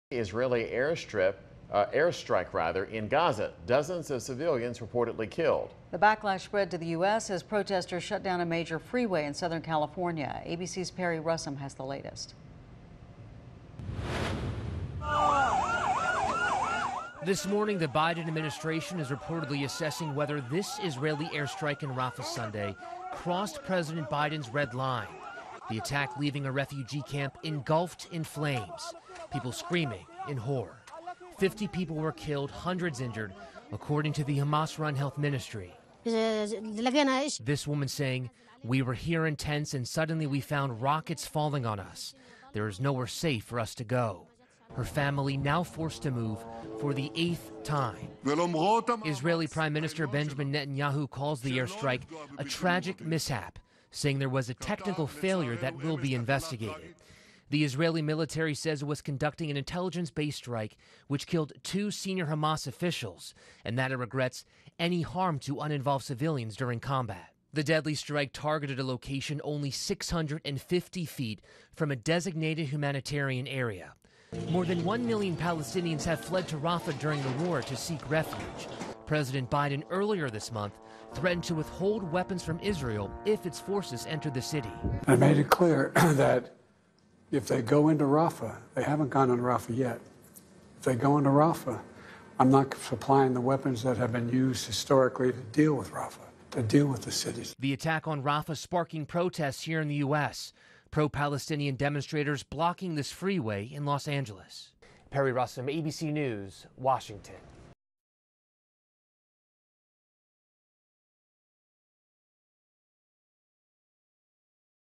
Israel-Hamas War: Netanyahu acknowledges 'tragic mistake' after Rafah strike kills dozens
Prime Minister Benjamin Netanyahu said Monday that a 'tragic mistake' was made in an Israeli strike in the southern Gaza city of Rafah that set fire to a camp housing displaced Palestinians.